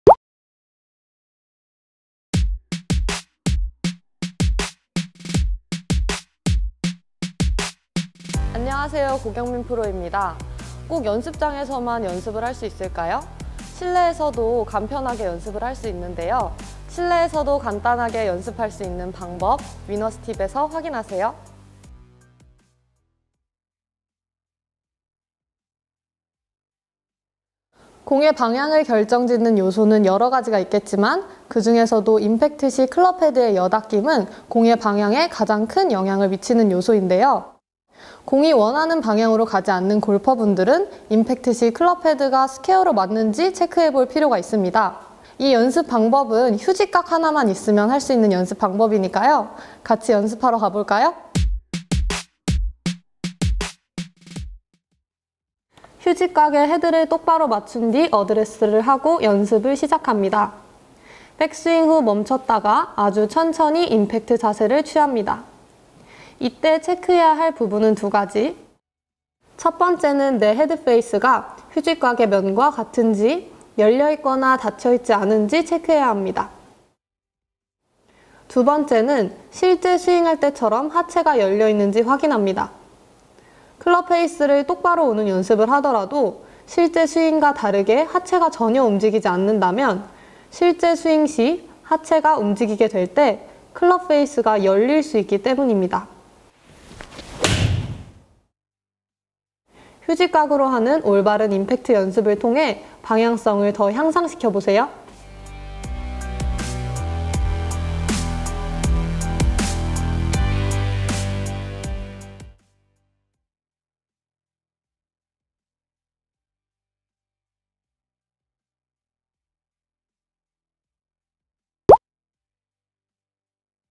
[위너스팁]④임팩트 연습 체크 포인트
"클럽헤드의 열림과 닫힘은 공의 방향을 결정짓는 중요한 요소다...중요한 것은 실제 스윙할 때처럼 하체를 움직이면서..."

***골프연습, 실내에서 효과적으로 할 수 있을까?

고경민 프로가 집 안에서도 간편하게 골프실력을 향상시킬 수 있는 연습방법을 소개합니다.

주변에서 쉽게 찾을 수 있는 도구를 활용해 실내에서도 효과적으로 골프 연습을 해볼까요?